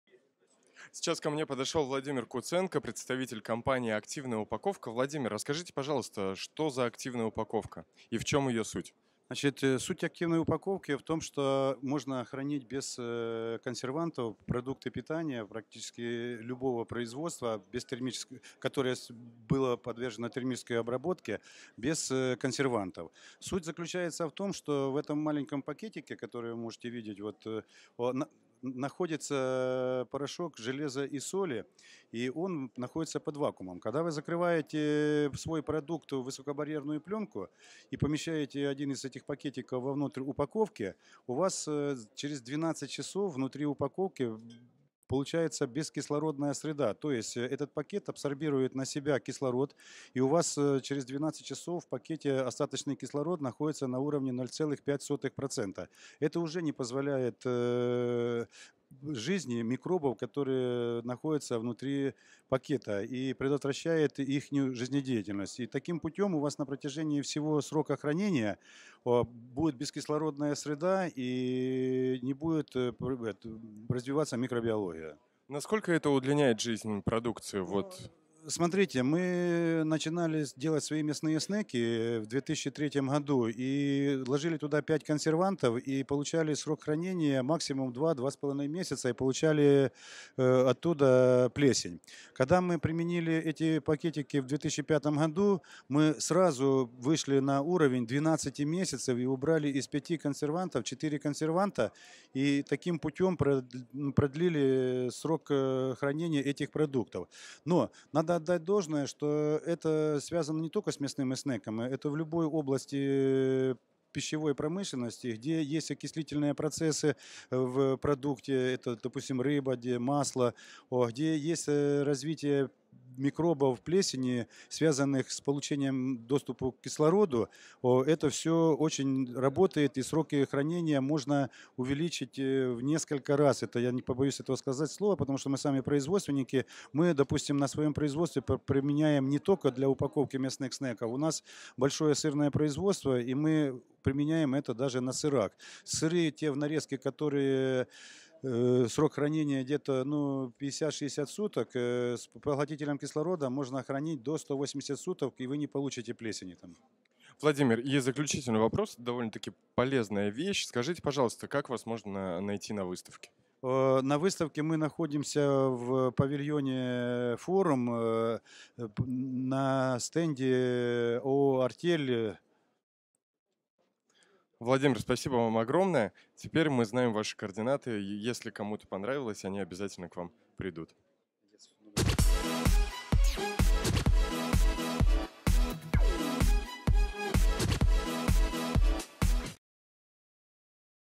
Владимир Куценко, Активная упаковка на
Рассказал о "волшебной" упаковке, которую производит компания Активная упаковка. Как продлить срок хранения продуктов в несколько раз?
Узнайте подробности и координаты стенда Активной упаковки с помощью видеоинтервью.